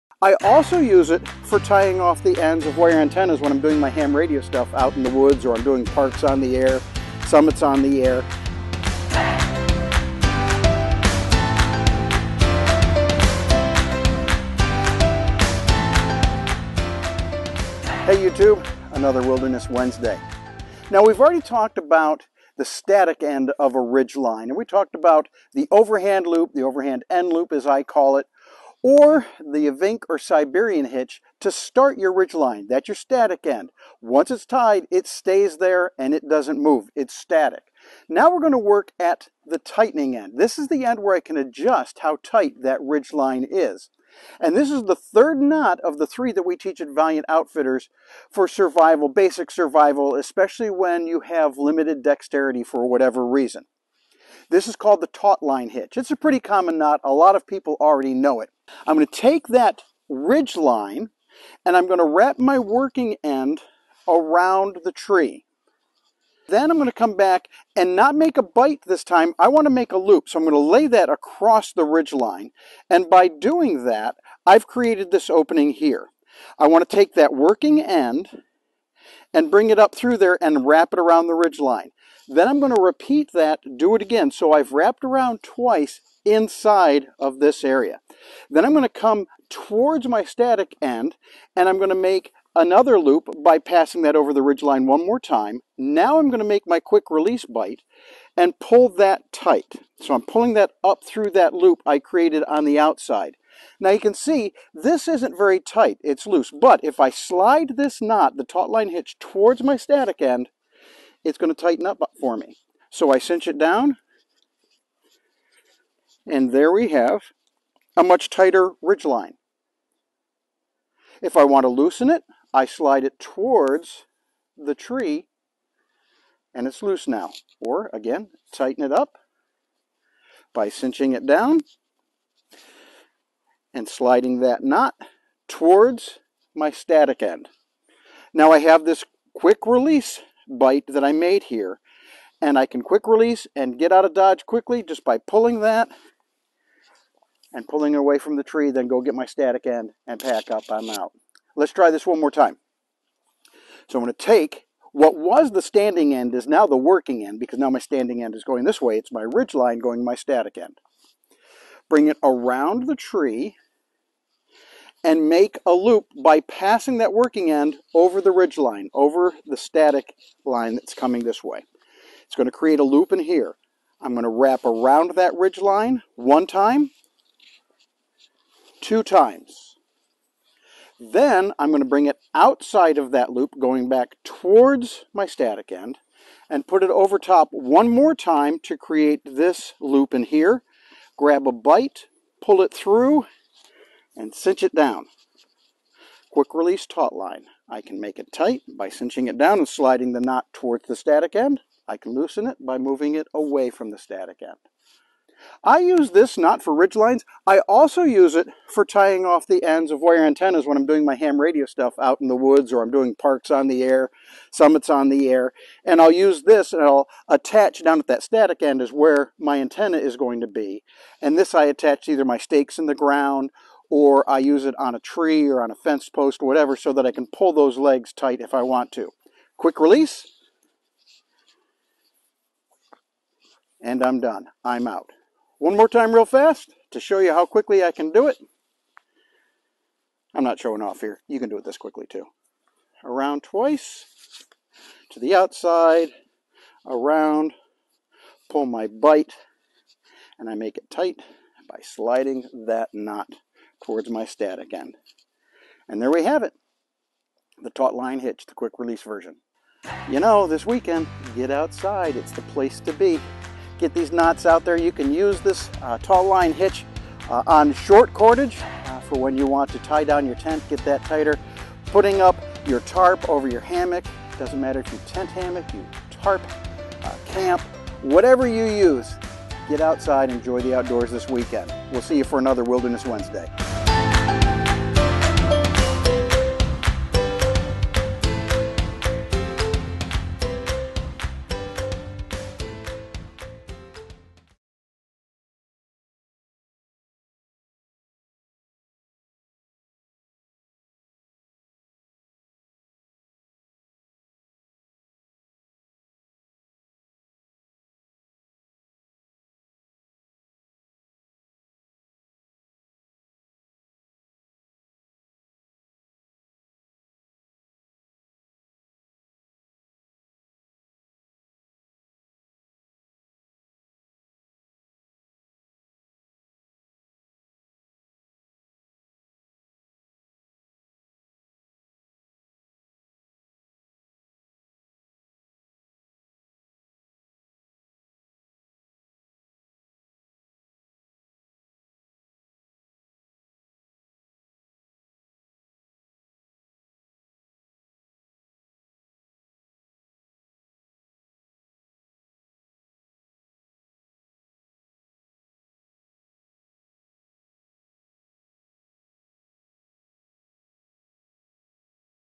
The Best Tensioning Knot for Survival - The Tautline Hitch
Keep your ridgeline and other tie-downs tight and secure with my favorite knot, the Tautline Hitch.